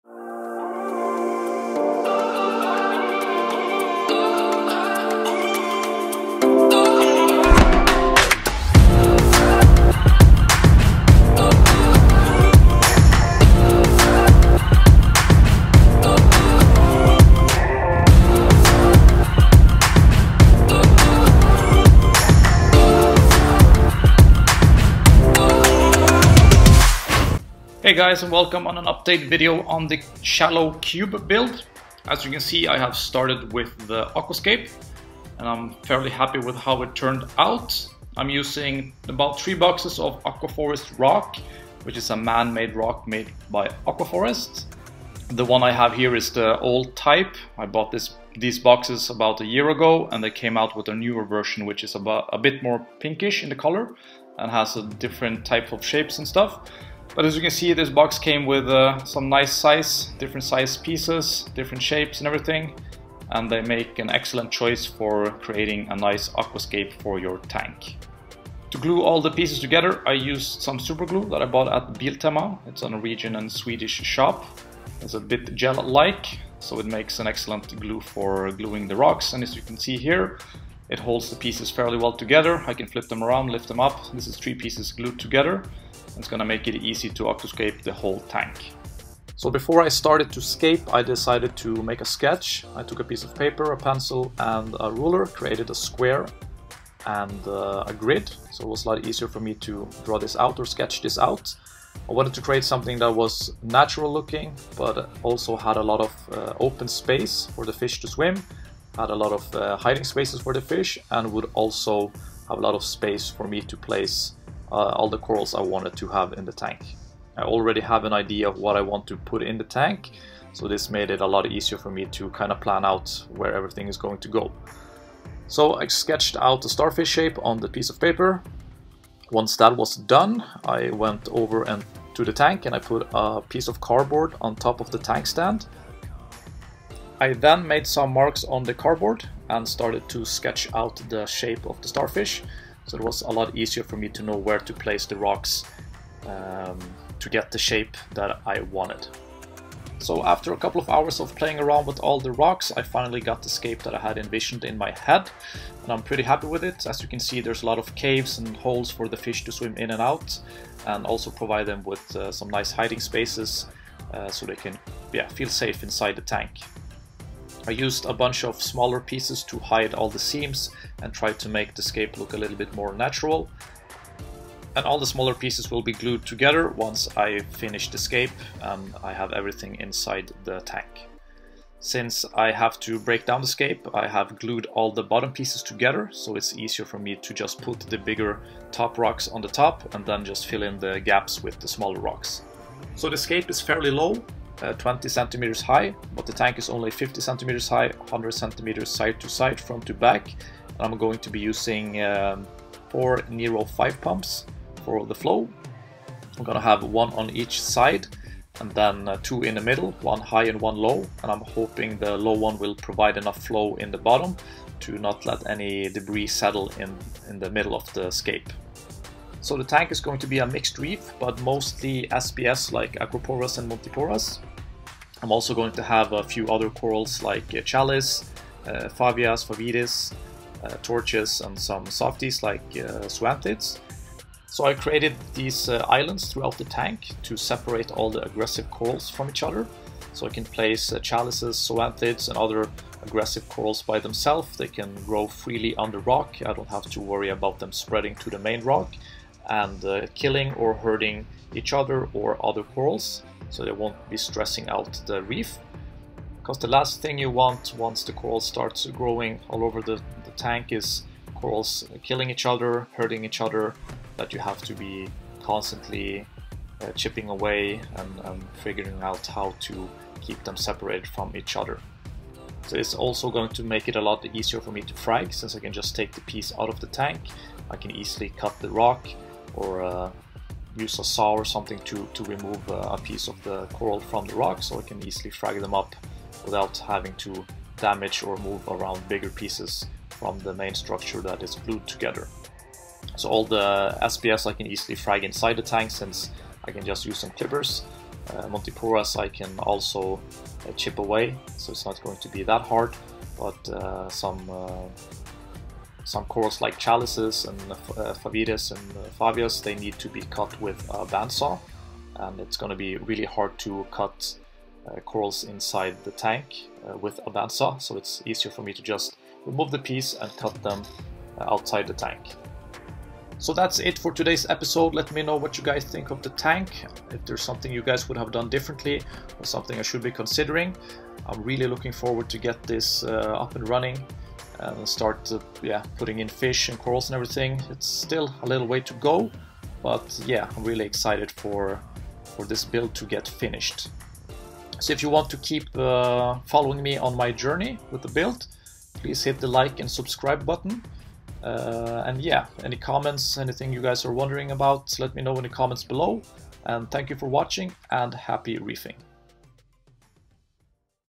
CoralLab Shallow Cube build - EP#2
Quick video on the Aquascape of our new living rom Display!